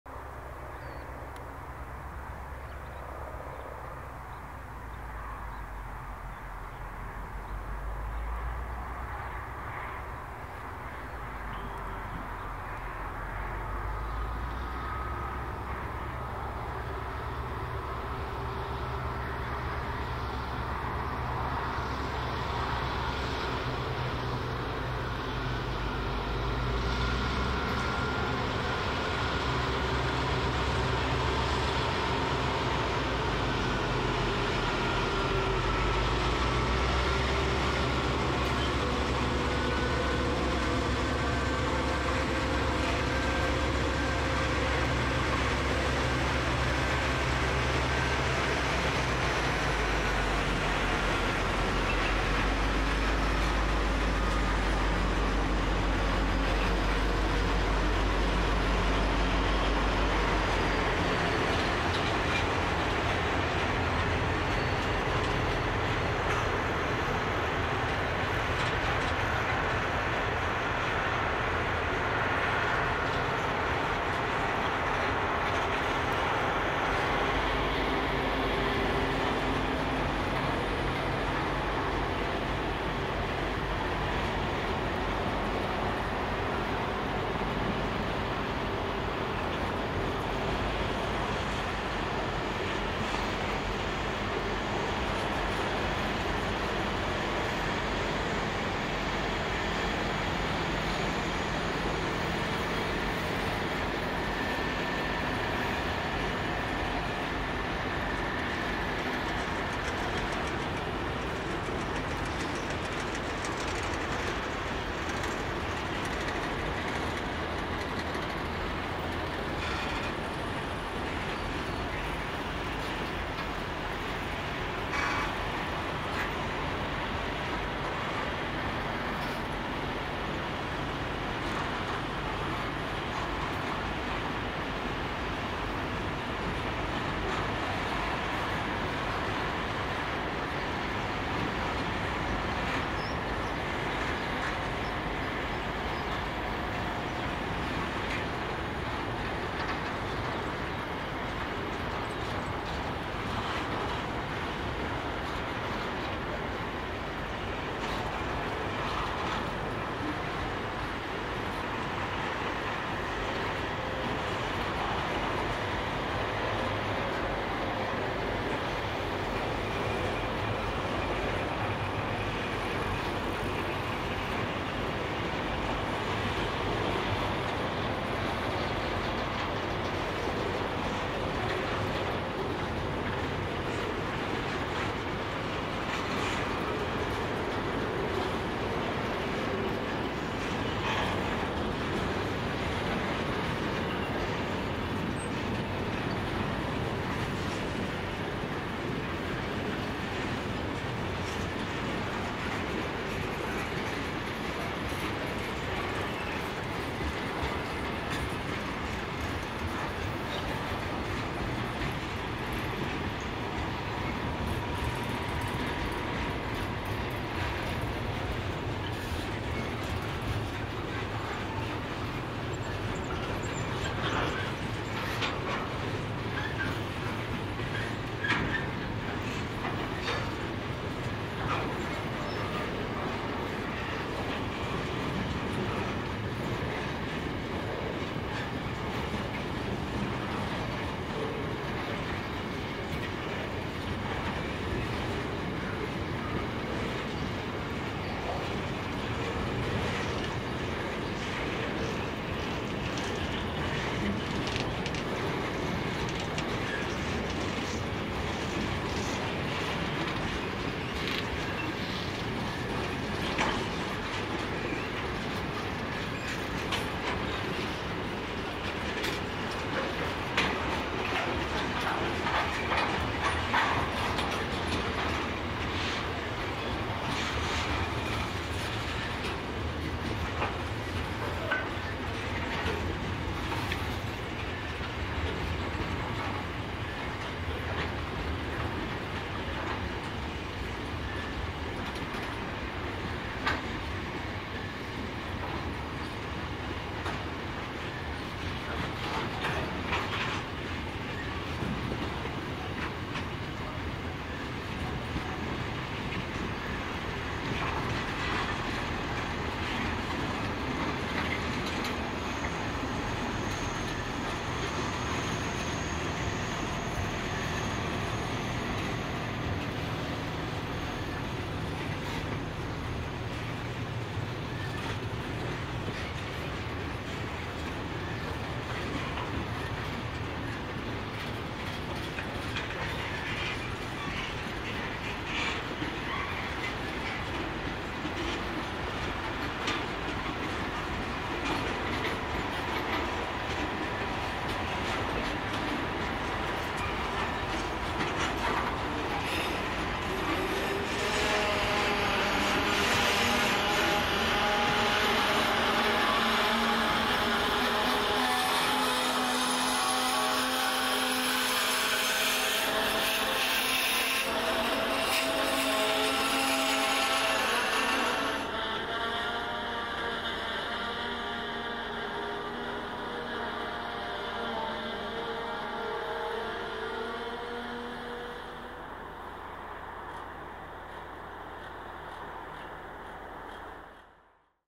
6MP4 Pacific National TOLL Container Train Enters The Loop Line At Gheringhap (21/6/2024) - PoathTV
Pacific National container service 6MP4 makes its way into the loop line in preparation for a cross with 7902V. Most of the loading on this train originates from a contract to haul containers for the TOLL group.

Locomotives / Consist: NR98 + NR31 + G526, 69 wagons.